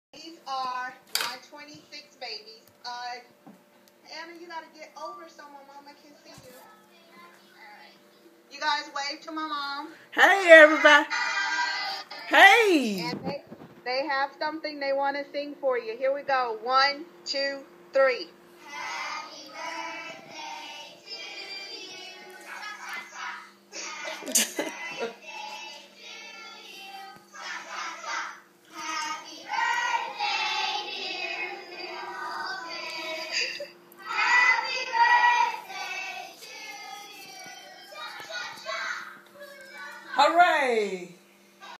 Birthday Greetings For Mom
Today is my mother Mrs. Carolyn Walton's Birthday.  My sister Katina Walton's 4th Grade Class is a fine example of how "technology
In the classroom works." We are truly a world without borders.